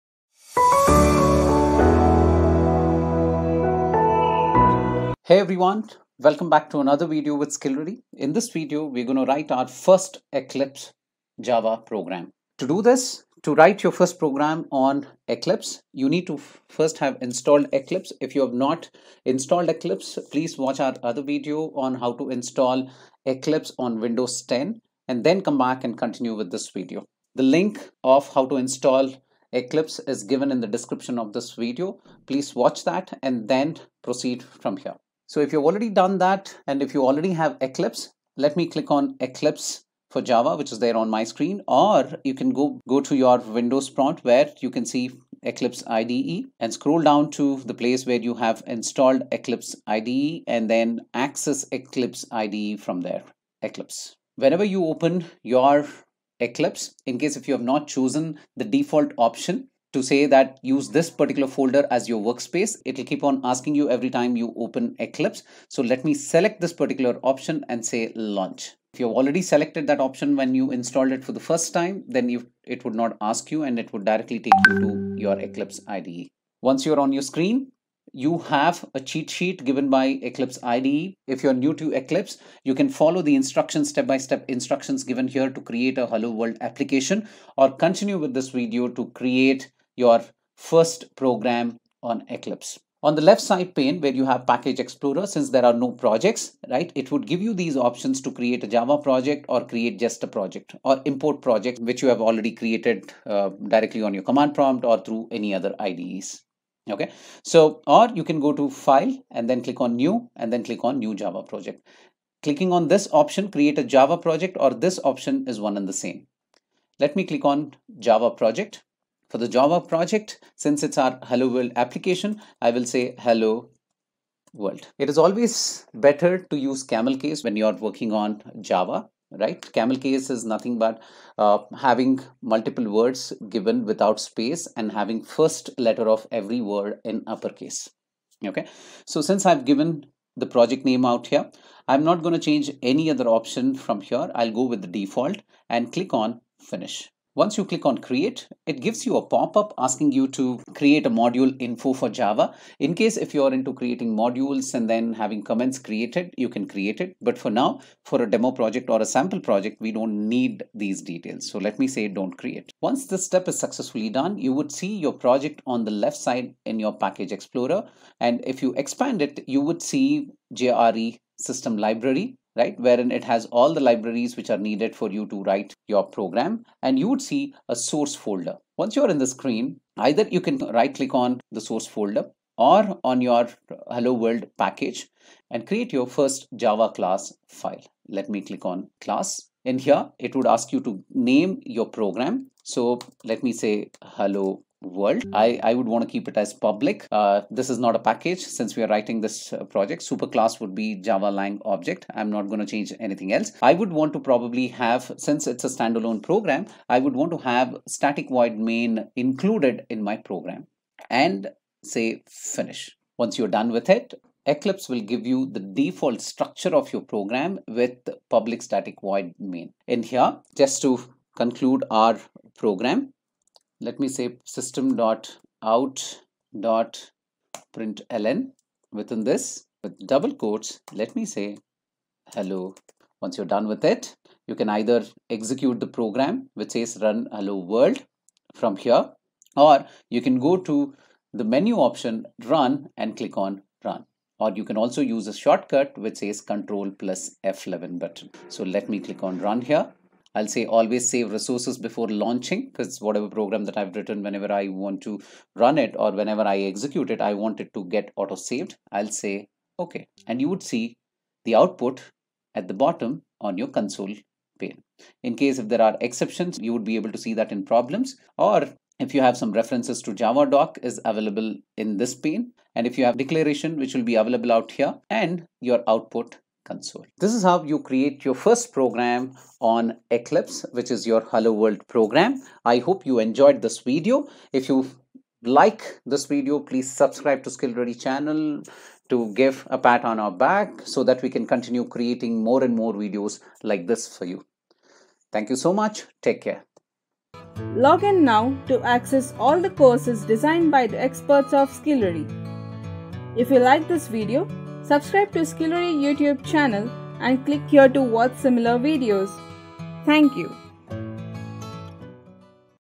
Write your first program on Eclipse | It can never get this easy | HelloWorld Program
This Tutorial clearly explains on How to Write a Simple Java Program using Eclipse

To write a "Hello World" program follow these steps:

Start Eclipse.
Create a new Java Project:
File-New-Project.
Select "Java" in the category list.
Select "Java Project" in the project list. Click "Next".
Enter a project name into the Project name field, for example, "Hello World Project".
Click "Finish"--It will ask you if you want the Java perspective to open. (You do.)
Create a new Java class:
Click the "Create a Java Class" button in the toolbar. (This is the icon below "Run" and "Window" with a tooltip that says "New Java Class.")
Enter "HelloWorld" into the Name field.
Click the checkbox indicating that you would like Eclipse to create a "public static void main(String[] args)" method.
Click "Finish".
A Java editor for HelloWorld.java will open. In the main method enter the following line.
     System.out.println("Hello World");
Save using ctrl-s. This automatically compiles HelloWorld.java.
Click the "Run" button in the toolbar (looks like a little man running).
You will be prompted to create a Launch configuration. Select "Java Application" and click "New".
Click "Run" to run the Hello World program. The console will open and display "Hello World".

For regular updates on the latest courses and assessments or certifications,